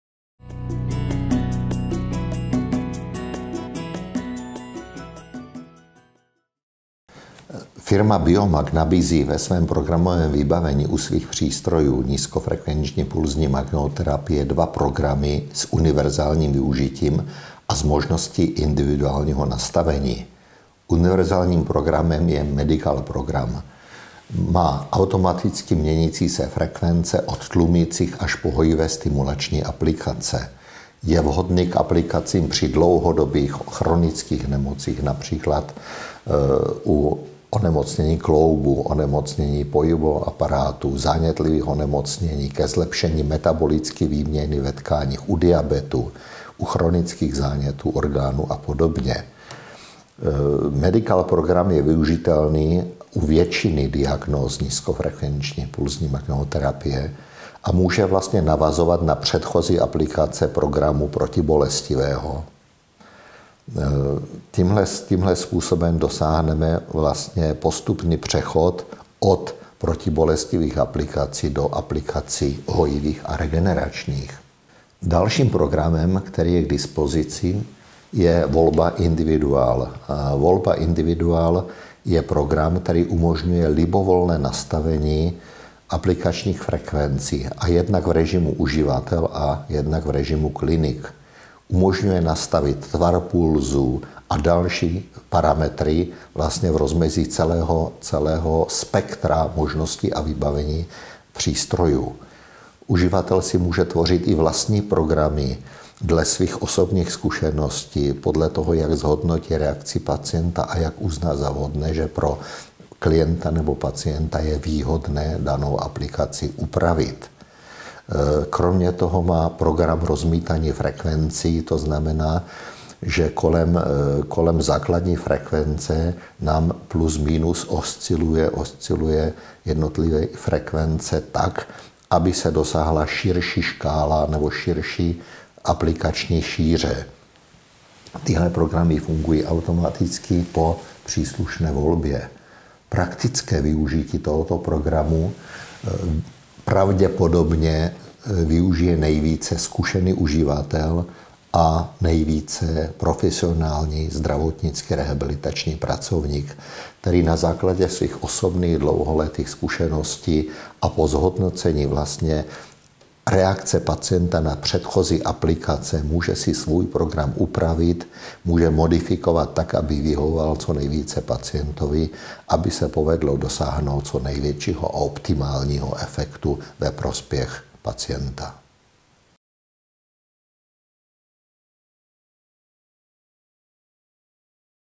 MUDr. Peter Bednarčík, CSc. - Účinky 3D pulzní magnetoterapie Biomag - programy Medical, Individual.
Tři desetiletí vývoje a inovací nejen jejich programového vybavení dávají přístrojům Biomag, využívajícím nízkofrekvenční pulzní magnetoterapii, schopnost pomoci při léčbě téměř stovky diagnóz. V rámci programu Medical si mohou pacienti snadným zvolením konkrétního léčebného programu nastavit terapii především na hojení, mají prokazatelný účinek také na tlumení bolesti. Program Individual zase umožňuje libovolné nastavení frekvencí, umožňuje tvořit si tak vlastní programy léčby nebo regenerace organismu.